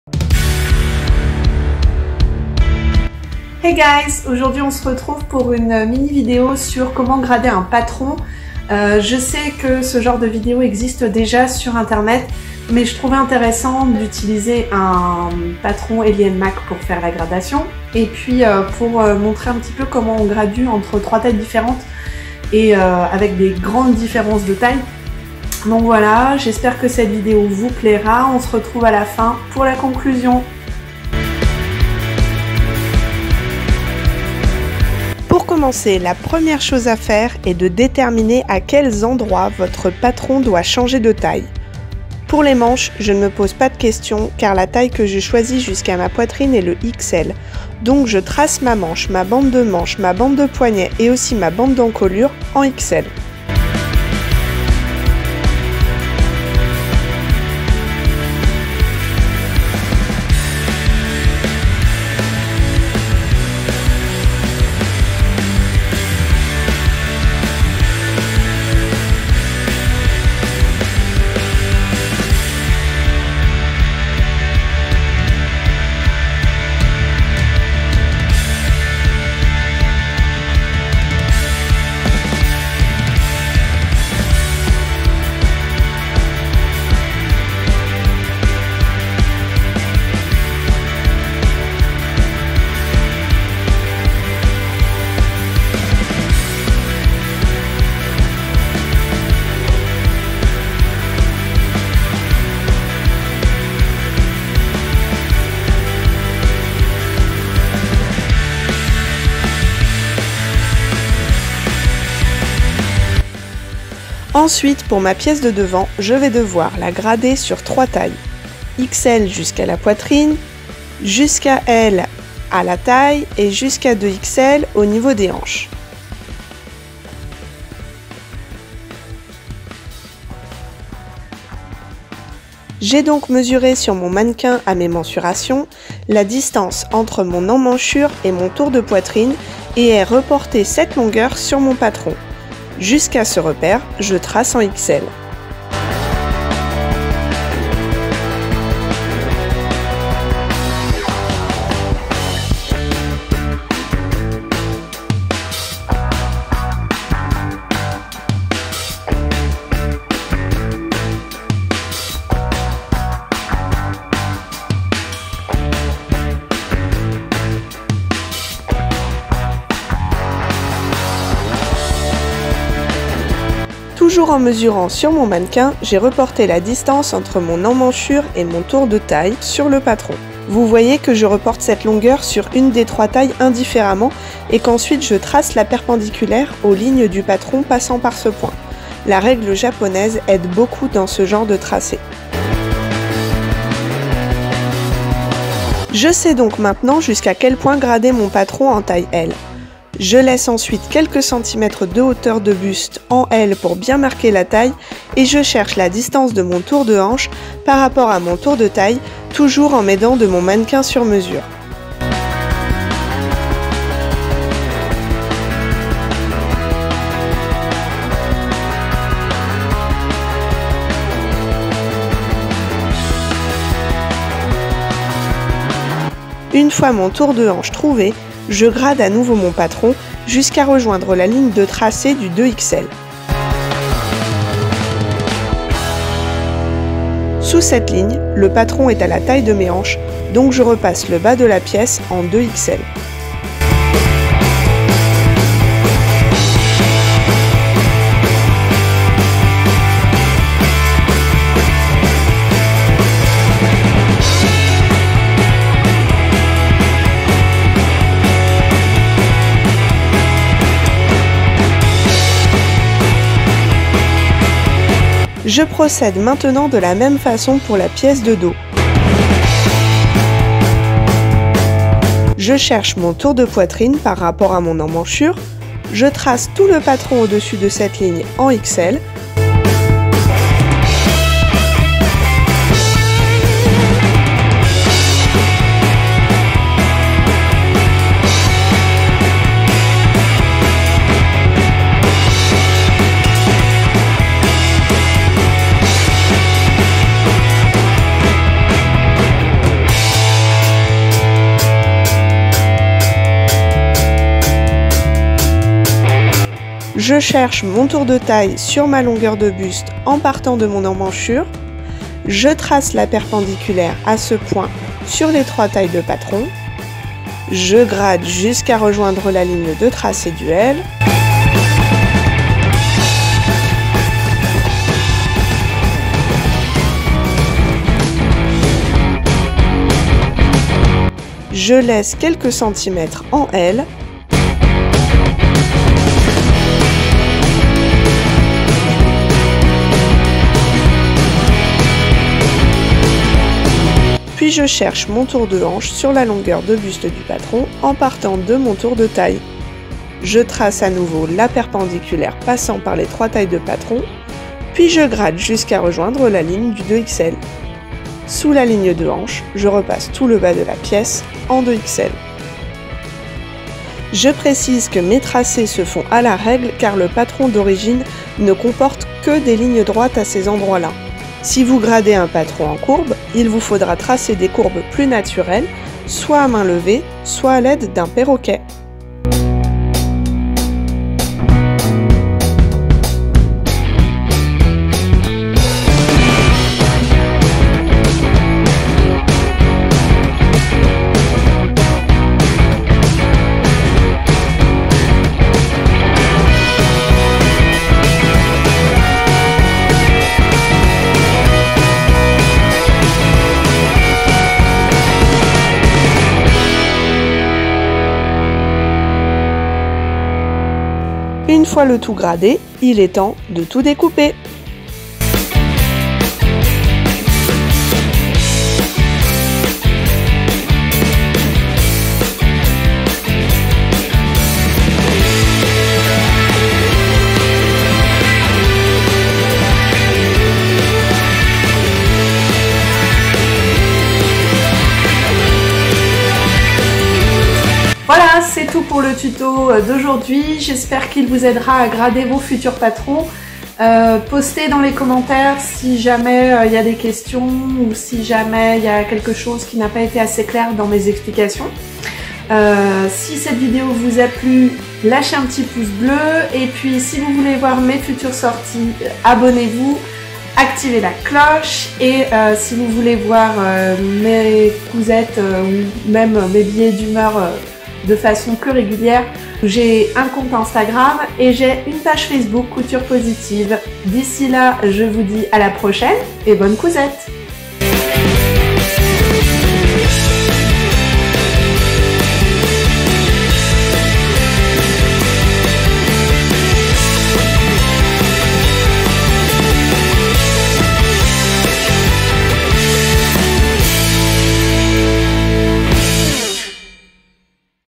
Les mini tutos - Episode 5 : Grader un patron
Dans cette vidéo, je vous explique comment je grade mes patrons entre trois tailles, à l'aide de mon mannequin sur mesure.